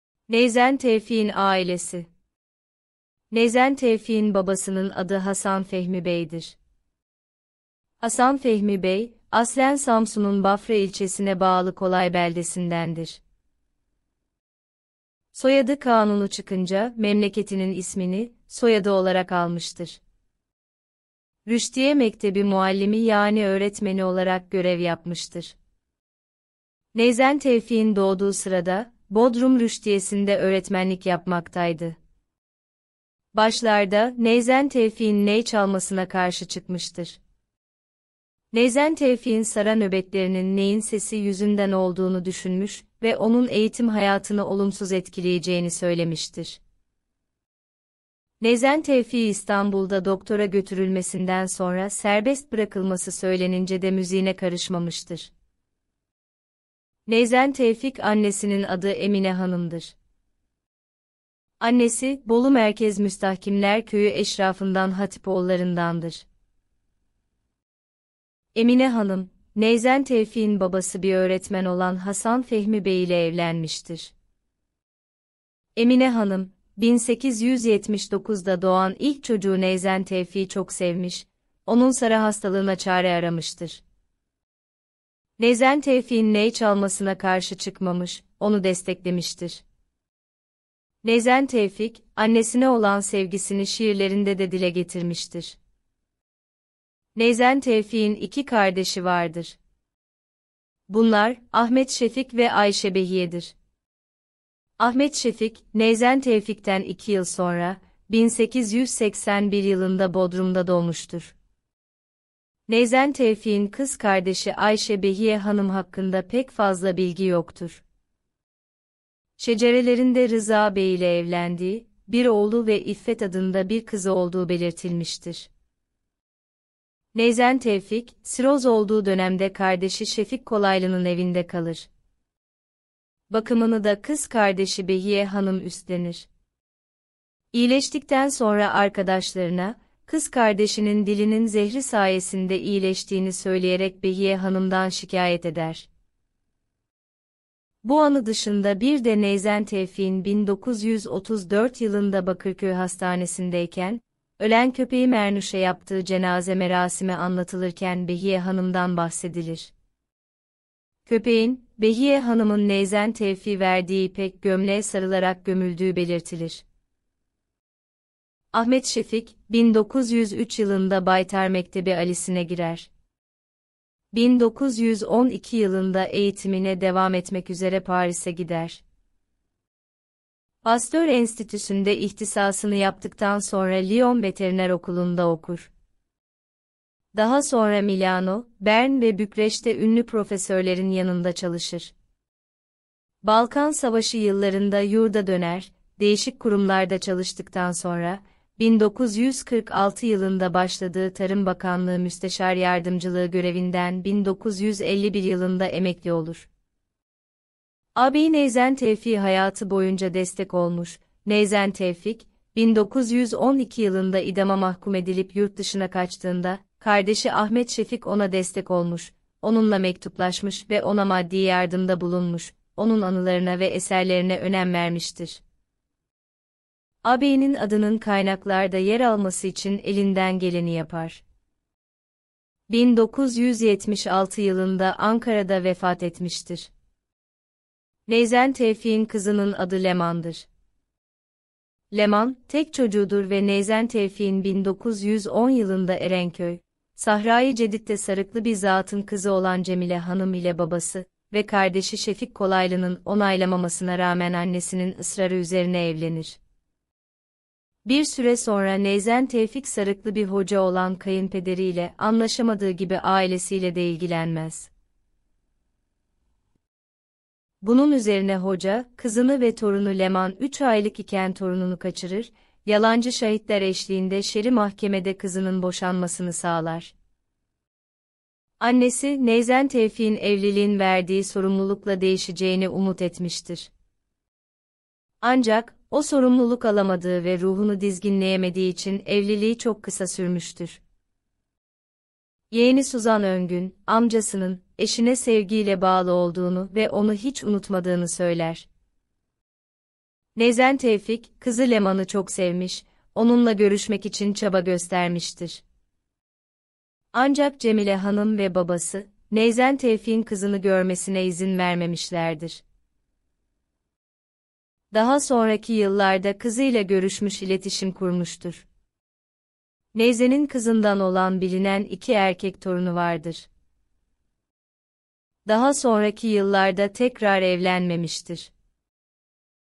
Neyzen Tevfik’in Ailesi.
Neyzen Tevfik’in babasının adı Hasan Fehmi Bey 1842 doğumlu olub, ölüm tarihi 8 Ekim 1917 dir. Hasan Fehmi Bey, aslen Samsun’un Bafra ilçesine bağlı Kolay beldesinden Hafız Mustafa ve Zübeyde Canikoğlu çifitinin oğuludır. Soyadı kanunu çıkınca, memleketinin ismini, soyadı olarak almıştır.
Rüştiye Mektebi muallimi yani öğretmeni olarak görev yapmıştır. Neyzen Tevfik’in doğduğu sırada, Bodrum Rüştiye’sinde öğretmenlik yapmaktaydı. Başlarda, Neyzen Tevfik’in ney çalmasına karşı çıkmıştır. Neyzen Tevfik’in sara nöbetlerinin neyin sesi yüzünden olduğunu düşünmüş ve onun eğitim hayatını olumsuz etkileyeceğini söylemiştir. Neyzen Tevfik’i İstanbul’da doktora götürülmesinden sonra serbest bırakılması söylenincede müziğine karışmamıştır.
Neyzen Tevfik annesinin adı Emine Hanım 1857 doğumlu olub, ölüm tarihi 9 Teşrin-i sani 1930 dur. Annesi, Bolu merkez Müstahkimler Köyü eşrafından Hatipoğulları’ndandır. Emine Hanım, Neyzen Tevfik’in babası bir öğretmen olan Hasan Fehmi Bey ile evlenmiştir. Emine Hanım, 1879 da doğan ilk çocuğu Neyzen Tevfik’i çok sevmiş, onun sara hastalığına çare aramıştır. Neyzen Tevfik’in ney çalmasına karşı çıkmamış, onu desteklemiştir. Neyzen Tevfik, annesine olan sevgisini şiirlerinde de dile getirmiştir.
Neyzen Tevfik’in iki kardeşi vardır. Bunlar, Ahmet Şefik ve Ayşe Behiye’dir. Ahmet Şefik, Neyzen Tevfik’ten iki yıl sonra, 1881 yılında Bodrum’da doğmuştur.  Neyzen Tevfik’in kız kardeşi Ayşe Behiye Hanım hakkında pek fazla bilgi yoktur. Şecerelerinde Rıza Bey ile evlendiği, bir oğlu ve İffet adında bir kızı olduğu belirtilmiştir.
Neyzen Tevfik, siroz olduğu dönemde kardeşi Ahmet Şefik Kolaylı’nın evinde kalır. Bakımını da kız kardeşi Ayşe Behiye Hanım üstlenir. İyileştikten sonra arkadaşlarına, kız kardeşinin dilinin zehri sayesinde iyileştiğini söyleyerek Behiye Hanım’dan şikâyet eder. Bu anı dışında bir de Neyzen Tevfik’in 1934 yılında Bakırköy Hastanesi’ndeyken, ölen köpeği Mernuş’a yaptığı cenaze merasimi anlatılırken Behiye Hanım’dan bahsedilir. Köpeğin, Ayşe Behiye Hanım’ın Neyzen Tevfik’e verdiği ipek gömleğe sarılarak gömüldüğü belirtilir.
Ahmet Şefik, 1903 yılında Baytar Mekteb-i Âlî’sine girer. 1912 yılında eğitimine devam etmek üzere Paris’e gider. Pastör Enstitüsü’nde ihtisasını yaptıktan sonra Lyon Veteriner Okulu’nda okur. Daha sonra Milano, Bern ve Bükreş’te ünlü profesörlerin yanında çalışır. Balkan Savaşı yıllarında yurda döner.Değişik kurumlarda çalıştıktan sonra, 1946 yılında başladığı Tarım Bakanlığı Müsteşar Yardımcılığı görevinden 1951 yılında emekli olur. Ağabeyi Neyzen Tevfik’e hayatı boyunca destek olmuş, Neyzen Tevfik, 1912 yılında idama mahkûm edilip yurtdışına kaçtığında,  kardeşi Ahmet Şefik ona destek olmuş, onunla mektuplaşmış ve ona maddi yardımda bulunmuş, onun anılarına ve eserlerine önem vermiştir. Ağabeyinin adının kaynaklarda yer alması için elinden geleni yapar. 1976 yılında Ankara’da vefat etmiştir.
Neyzen Tevfik’in kızının adı Leman’dır. Leman, tek çocuğudur ve Neyzen Tevfik’in 1910 yılında Erenköy, Sahra-yı Cedit’te sarıklı bir zatın kızı olan Cemile Hanım ile babası ve kardeşi Şefik Kolaylı’nın onaylamamasına rağmen annesinin ısrarı üzerine evlenir. Bir süre sonra Neyzen Tevfik sarıklı bir hoca olan kayınpederi ile anlaşamadığı gibi ailesiyle de ilgilenmez. Bunun üzerine hoca, kızını ve torunu Leman üç aylık iken torununu kaçırır, yalancı şahitler eşliğinde şer’i mahkemede kızının boşanmasını sağlar.  Annesi, Neyzen Tevfik’in evliliğin verdiği sorumlulukla değişeceğini umut etmiştir. Ancak, o sorumluluk alamadığı ve ruhunu dizginleyemediği için evliliği çok kısa sürmüştür. Yeğeni Suzan Öngün, amcasının, eşine sevgiyle bağlı olduğunu ve onu hiç unutmadığını söyler. Neyzen Tevfik, kızı Zübeyde Leman’ı çok sevmiş, onunla görüşmek için çaba göstermiştir. Ancak Cemile Hanım ve babası, Neyzen Tevfik’in kızını görmesine izin vermemişlerdir. Neyzenin kızından olan bilinen iki erkek torunu vardır. Daha sonraki yıllarda tekrar evlenmemiştir.